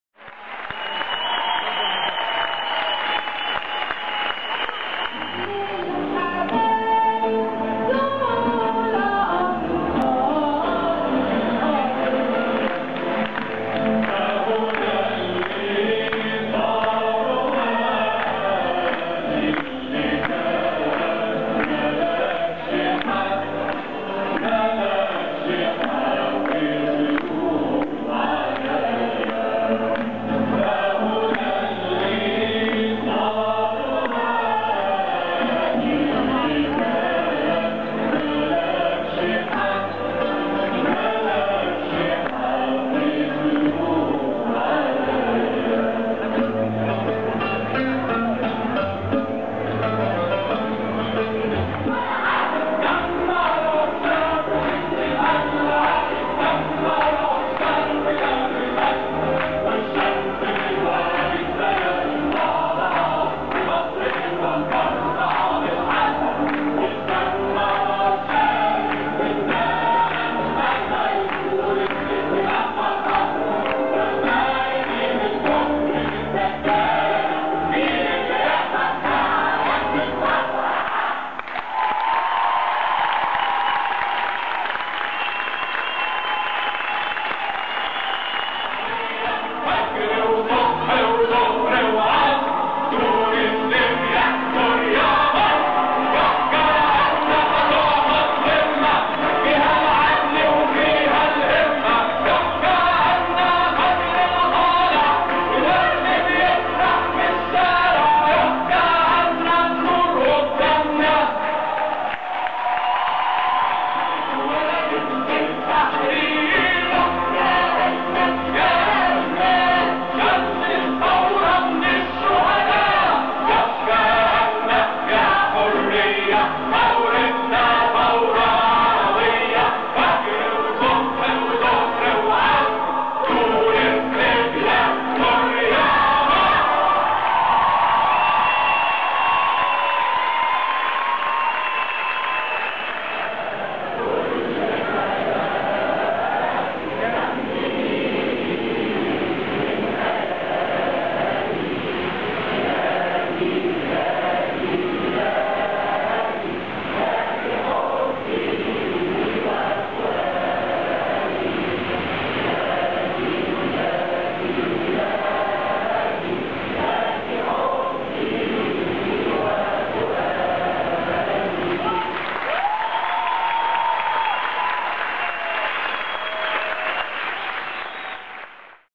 Eskenderella at El Sawy Culturewheel (mix)
An extremely popular band! During the Januar 25 Revolution, they performed in the middle of Tahrir square.